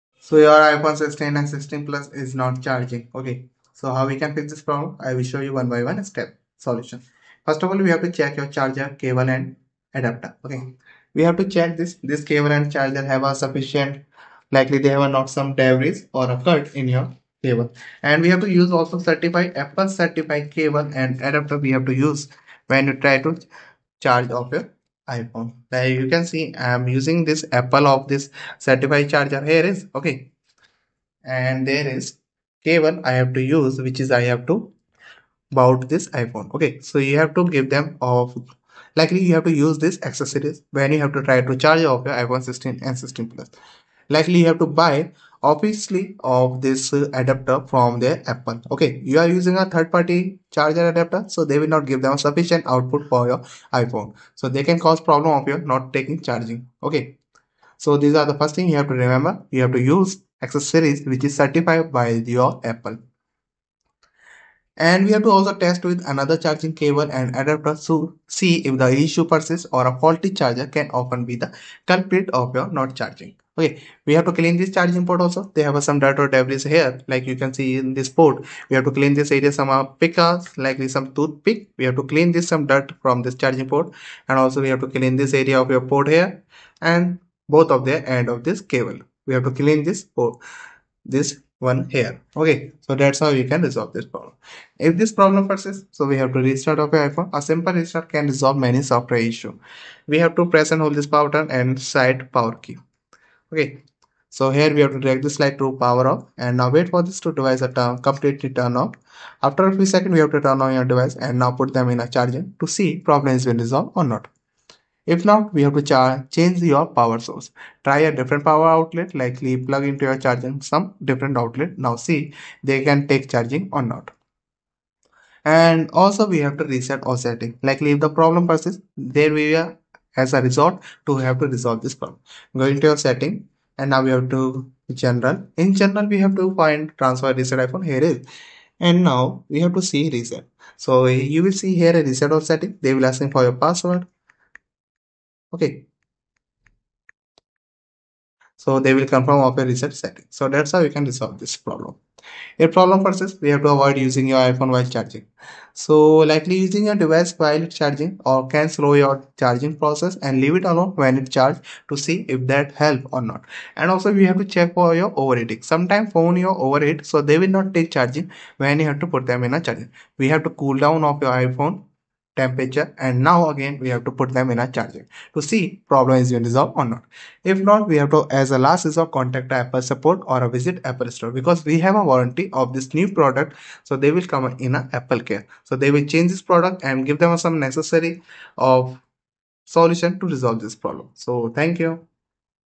iPhone 16/16 Plus Battery Not Charging: Here’s How To Fix
In this informative video, we address a common issue faced by iPhone 16 and iPhone 16 Plus users: the battery not charging.

00:10 FIX 1 : CHECK ACCESORIIES
01:05 FIX 2 : CHANGE ACCESORIES
01:58 FIX 3 : TRY DIFFERNT POWER OUTLET
02:05 FIX 4 : RESET